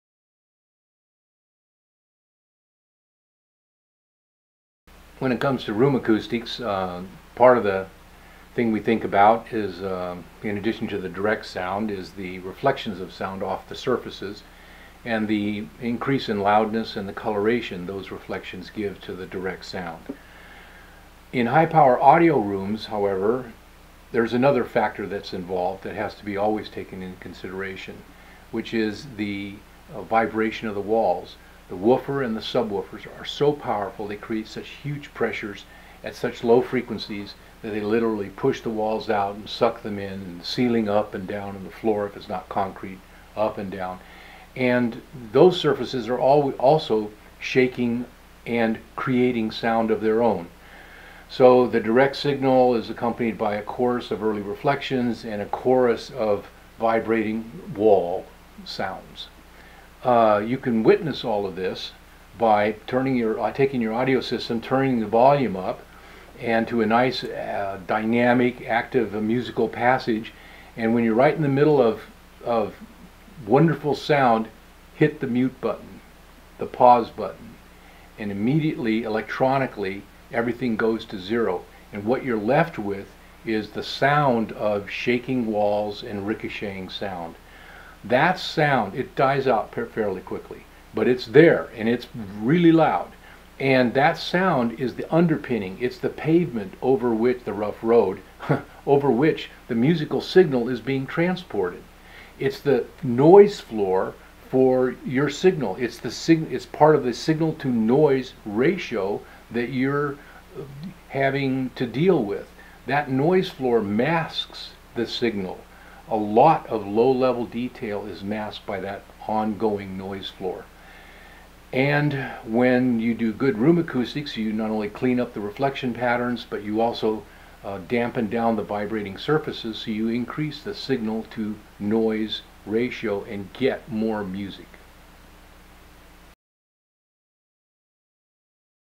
Art Noxon Answers: How Can You Test Your Room's Noise Floor?
Art Noxon, Acoustic Engineer and President of Acoustic Sciences Corporation, provides an easy way to hear your room's noise floor.

Why not stop by our website too? You can learn more about our products, and order some as well.